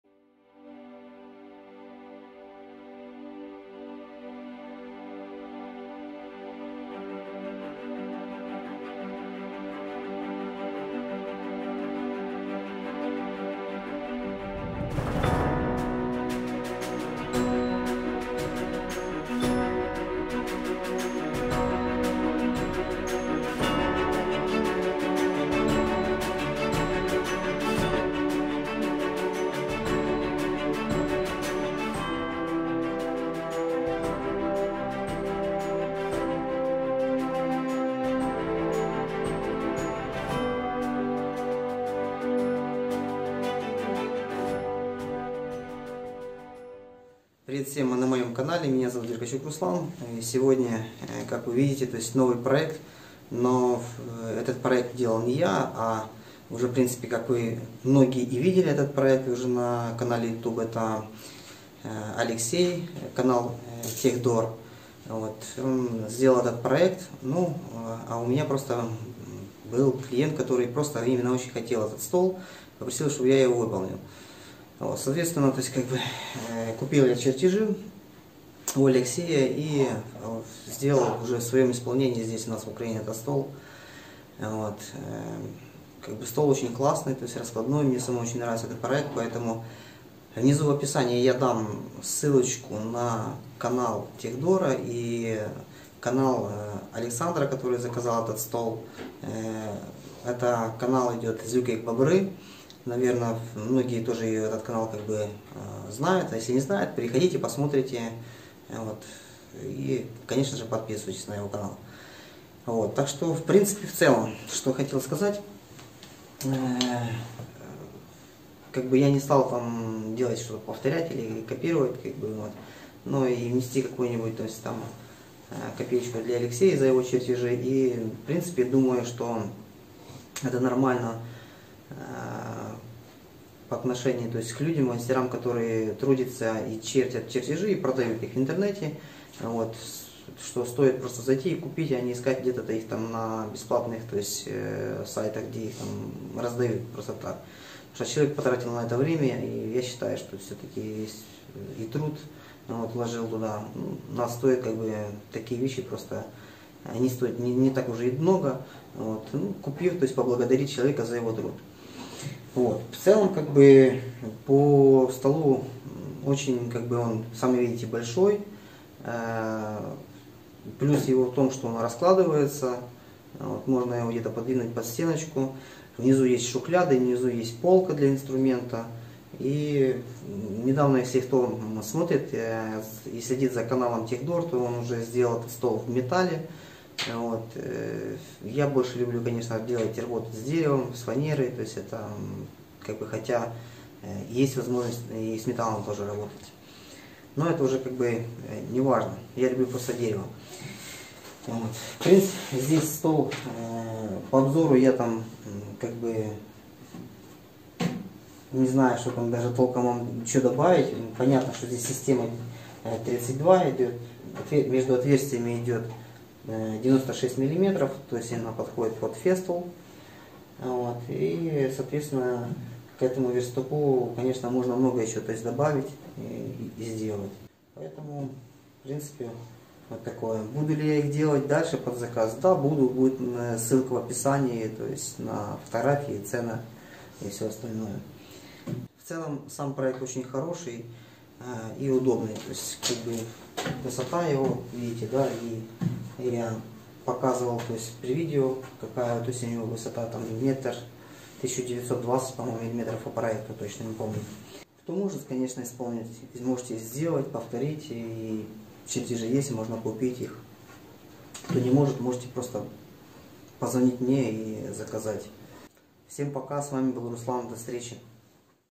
Складной верстак
Сделал под заказ по проекту от Алексея канал Tehdoor